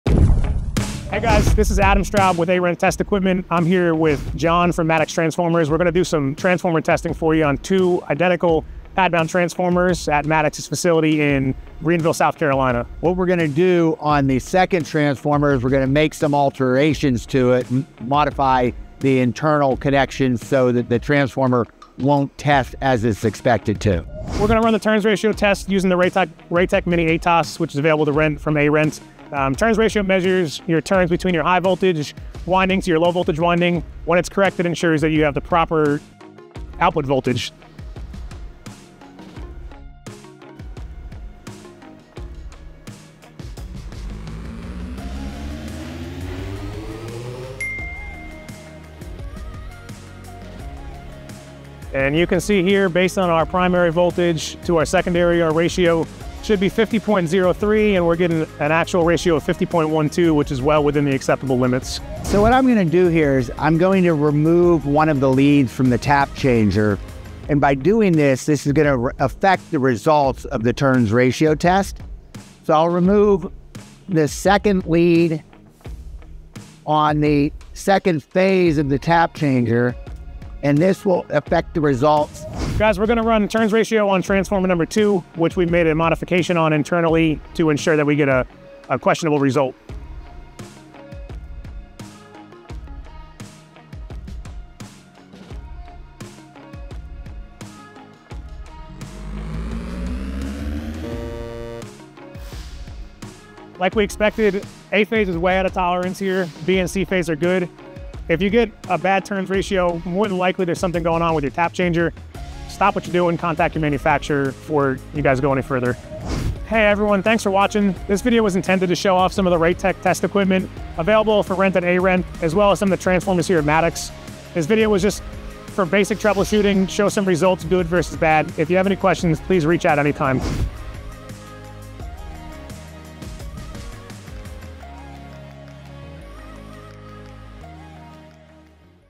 Transformer Turns Ratio Testing | Maddox Transformer & A-Rent
Transformer Turns Ratio Testing.

We've partnered with A-Rent Test Equipment to bring you this example of a TTR test. One transformer is connected properly; on the other, one of the tap leads is removed. See how the test results differ!

Timestamps:
0:00 Introduction
0:24 First turns ratio test
0:52 Test results
1:02 Adjusting the transformer
1:23 Second turns ratio test
1:43 Test results
1:57 Conclusion